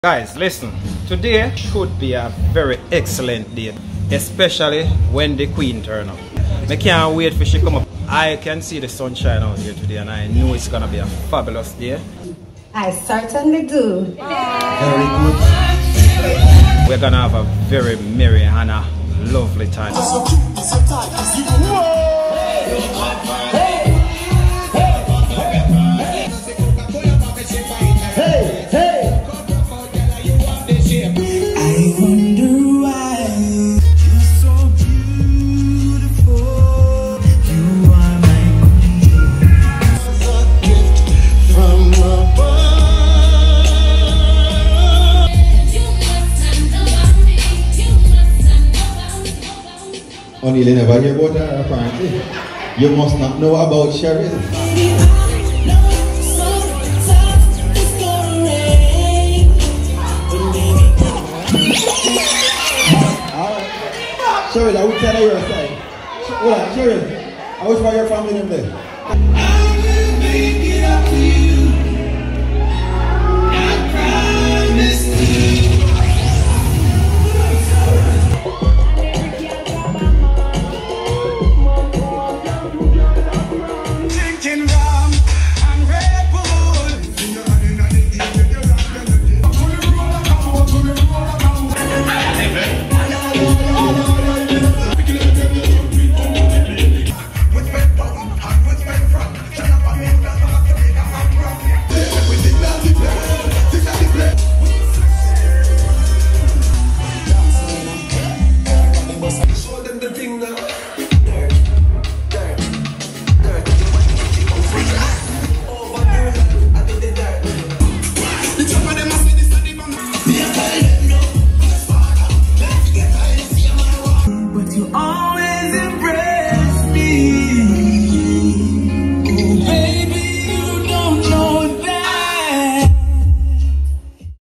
Jamaican Wedding 0 Cheryl & Oniel ❤️ Caymanas Golf Club
heryl & Oniel Gayle ❤️❤️❤️

Cheryl said yes to Oniel "Mr. Festive" Gayle iduring their midday wedding ceremony at the in St. Catherine, Jamaica

876sounds provided: Audio / DJ, Dance floor with decal